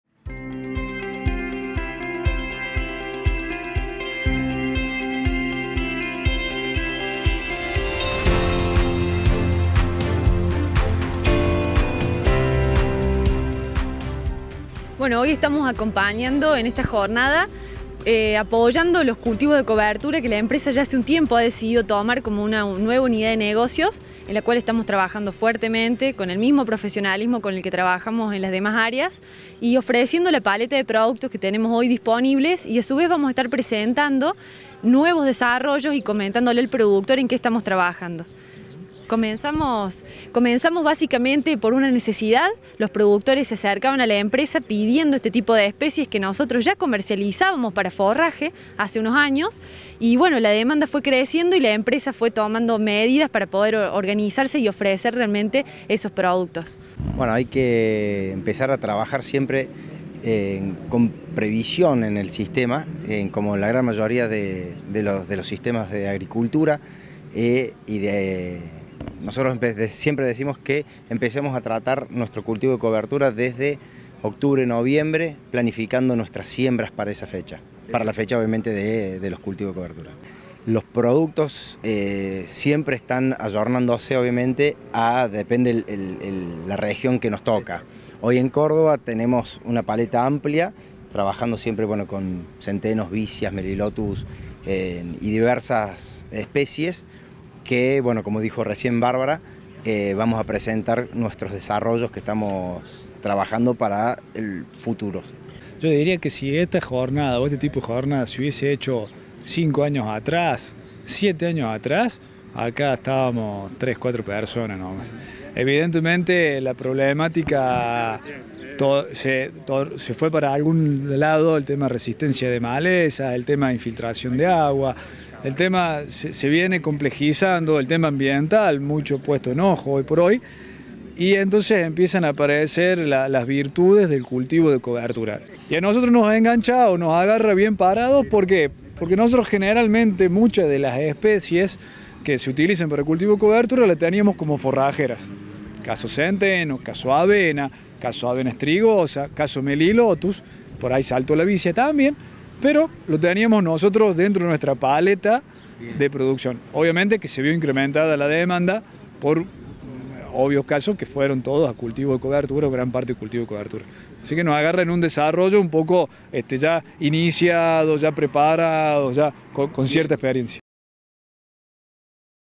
Oscar Pemán Semillas presente en la Primera Jornada de Cultivos de Cobertura
Participamos de la primera “Jornada de Cultivos de Cobertura de la Región Centro de Córdoba”. Informar, pensar y planificar, para la campaña 2020